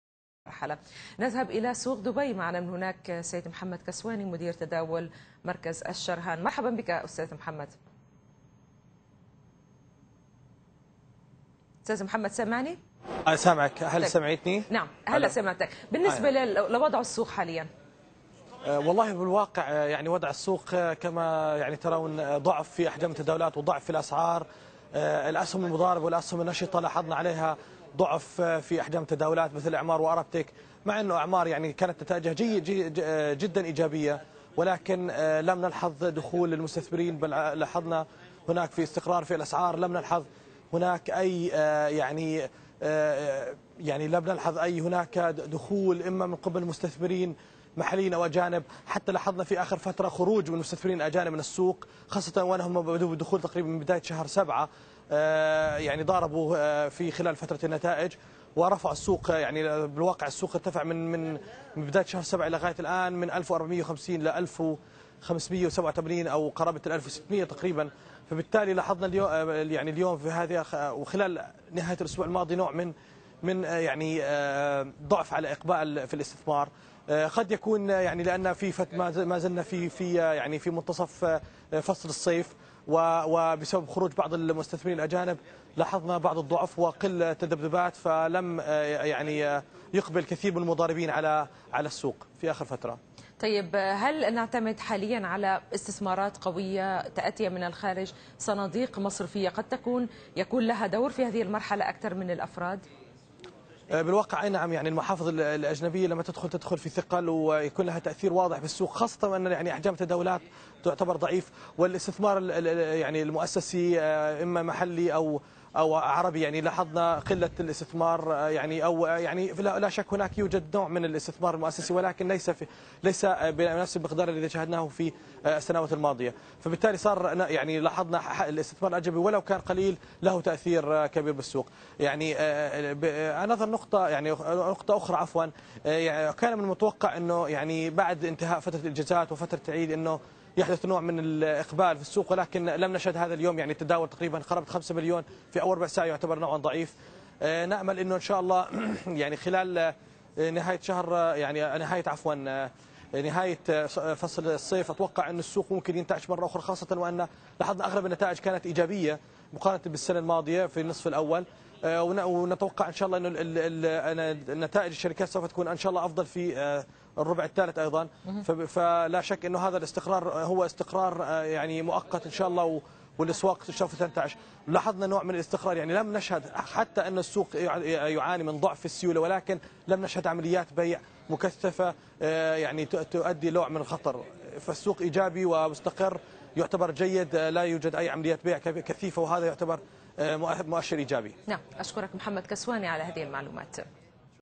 الخمول يسيطر على تداولات سوق دبي المالي
قال محمد الكسواني مدير مركز الشرهان للوساطة ان هناك ضعف في تداولات سوق دبي المالي و ضعف في الاسعارو السيولة يؤدي الى خمول مسيطر على اداء السوق اليوم و قد يرجع ذلك اننا مازلنا في منتصف فصل الصيف الذي يعرف فية قلة التداول و ايضا خروج بعض المستثمرين الاجانب